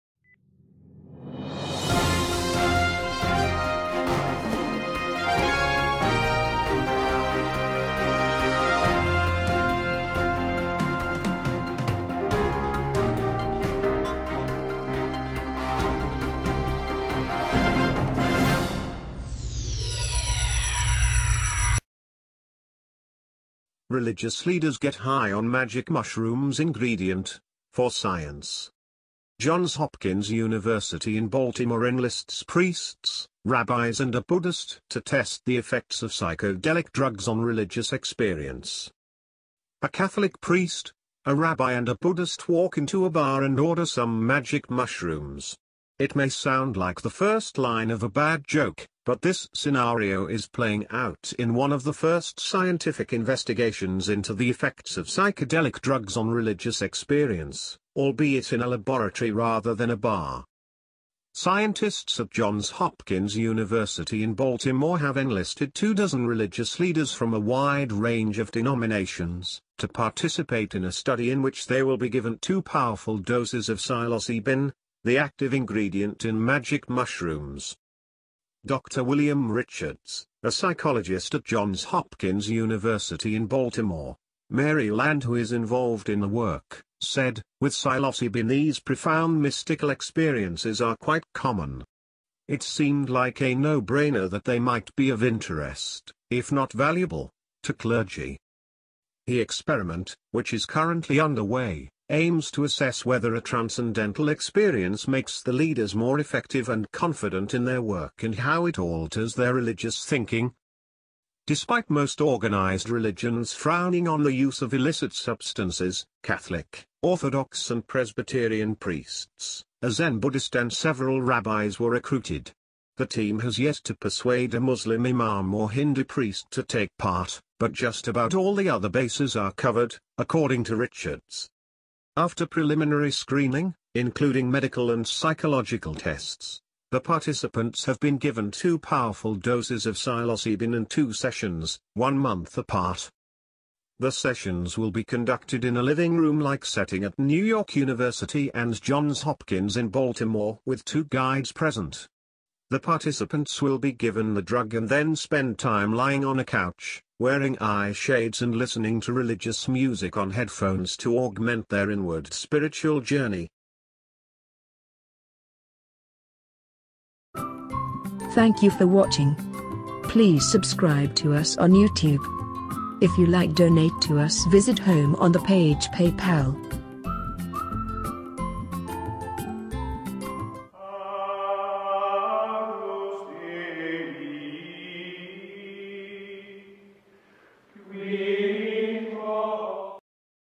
Religious leaders get high on magic mushrooms ingredient for science
Johns Hopkins University in Baltimore enlists priests, rabbis and a Buddhist to test the effects of psychedelic drugs on religious experience

A Catholic priest, a Rabbi and a Buddhist walk into a bar and order some magic mushrooms. It may sound like the first line of a bad joke, but this scenario is playing out in one of the first scientific investigations into the effects of psychedelic drugs on religious experience – albeit in a laboratory rather than a bar.

Scientists at Johns Hopkins University in Baltimore have enlisted two dozen religious leaders from a wide range of denominations, to participate in a study in which they will be given two powerful doses of psilocybin, the active ingredient in magic mushrooms.